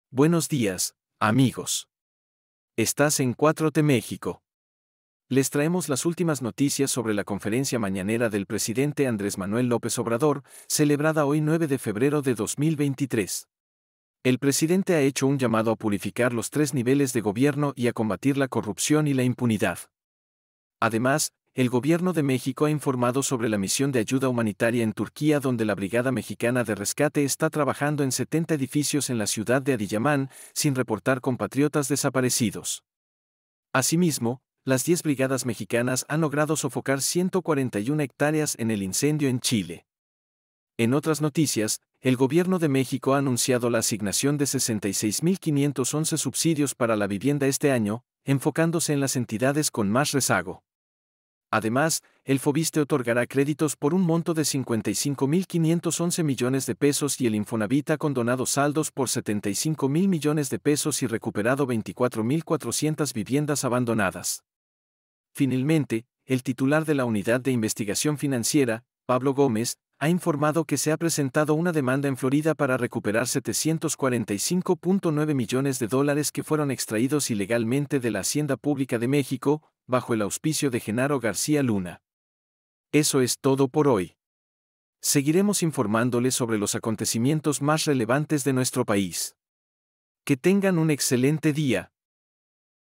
RESUMEN DE LA CONFERENCIA "MAÑANERA"
Resumen de la conferencia del Presidente López Obrador de este 09 de febrero de 2023.